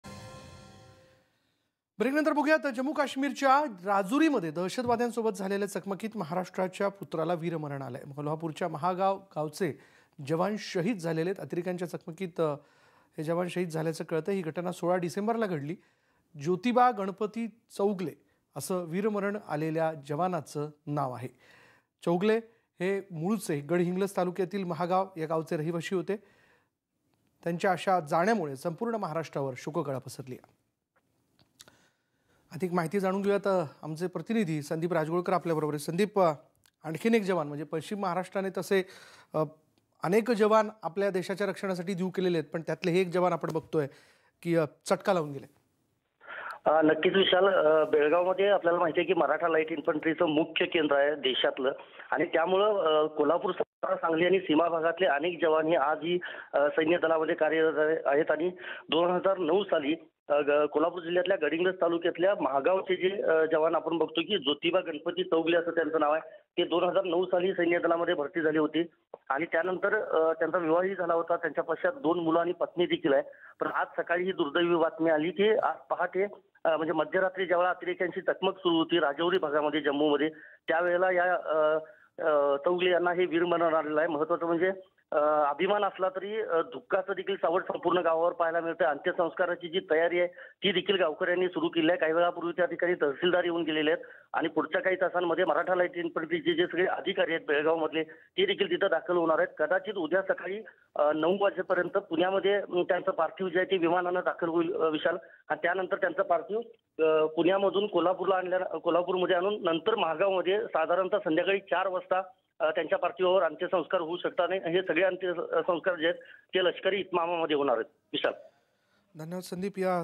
Jammu and Kashmir: दहशतवाद्यांशी लढताना कोल्हापूरच्या सुपुत्राला वीरमरण | News18 Lokmat
जम्मू काश्मीरच्या राजुरीमध्ये दहशतवाद्यांसोबत झालेल्या चकमकीत महाराष्ट्राच्या पुत्राला वीरमरण आलं आहे. कोल्हापूरच्या महागाव गावचे जवान शहीद अतिरेक्यांच्या चकमकीत शहीद झाल्याची घटना 16 डिसेंबरला घडली आहे. जोतिबा गणपती चौगले असं वीरमरण आलेल्या जवानाचं नाव आहे.